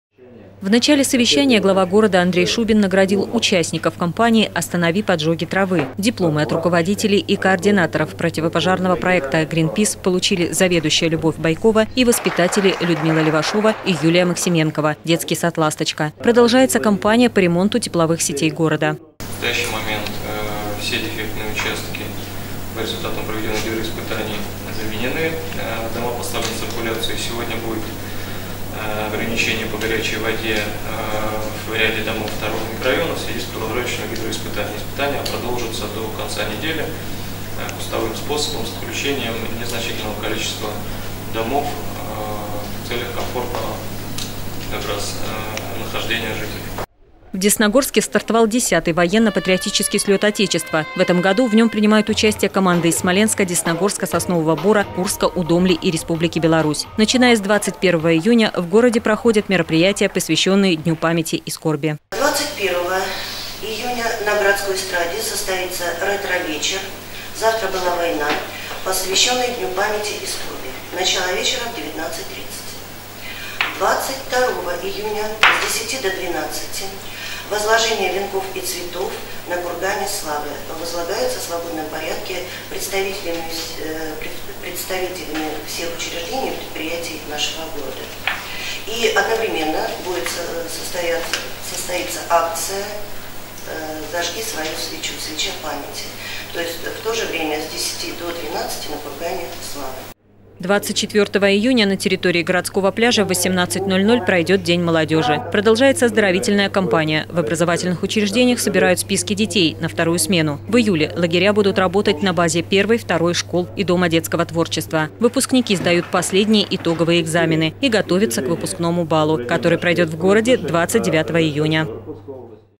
Десна-ТВ: Плановое оперативное совещание у Главы города
20-го июня в администрации прошло плановое оперативное совещание главы города с руководителями комитетов и муниципальных предприятий Десногорска. Подробности в нашем сюжете.